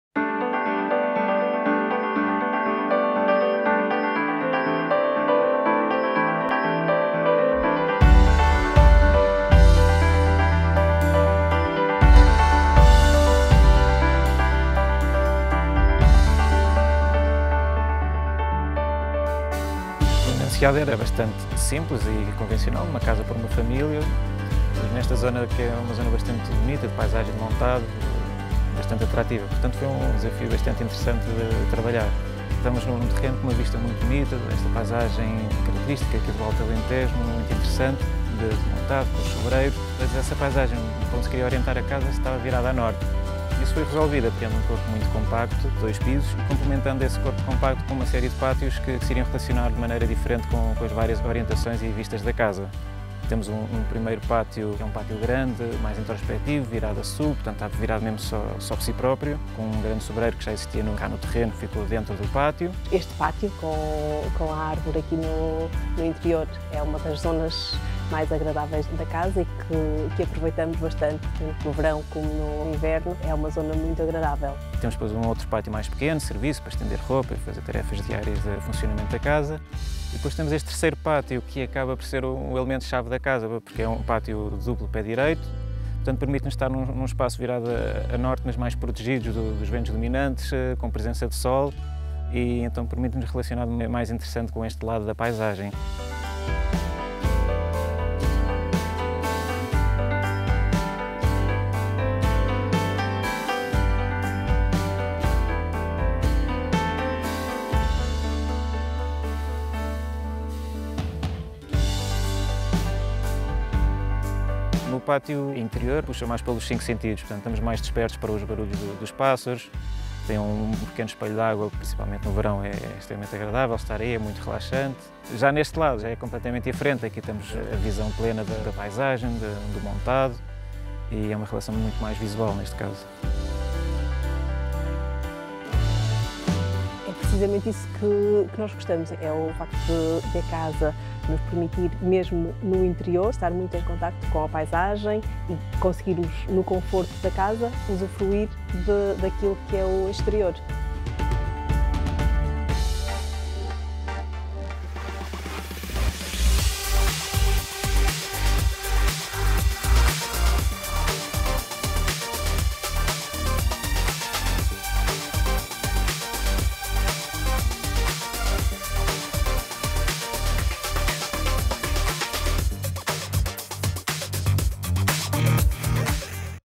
ARQ. 3 | CASA REPORT - Casa com Três Pátios, Miguel Marcelino
A paisagem de montado ditou a criação de três pátios, nesta casa que tenciona pousar no terreno, como se aí tivesse surgido, naturalmente. As linhas contemporâneas privilegiam a abertura das divisões, para um pleno convívio em família, com a Natureza.